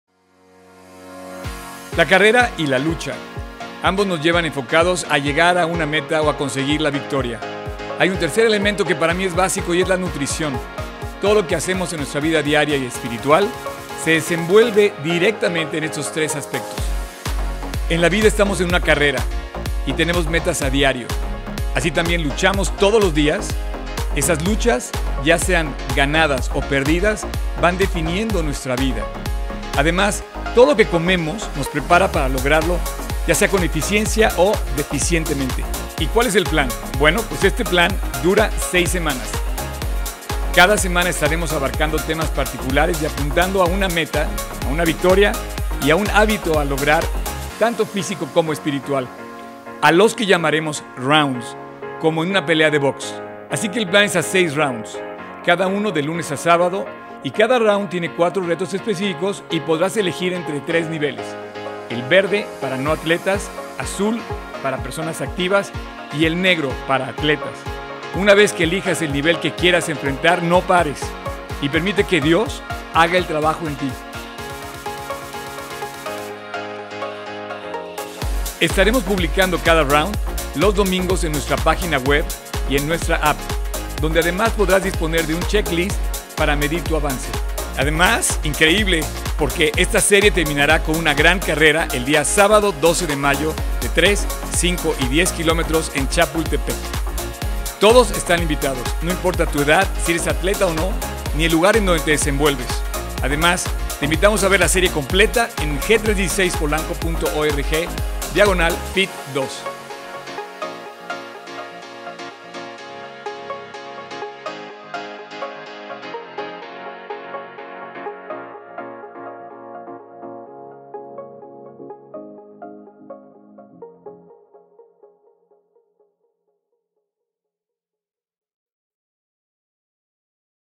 FIT v2.0
FITv2 es una serie que conecta la BIBLIA con el DEPORTE para que logres tus metas ESPIRITUALES y FÍSICAS. 😀 La CARRERA y la LUCHA conlleva una meta/victoria. Pero la NUTRICIÓN es básica para alcanzarlo.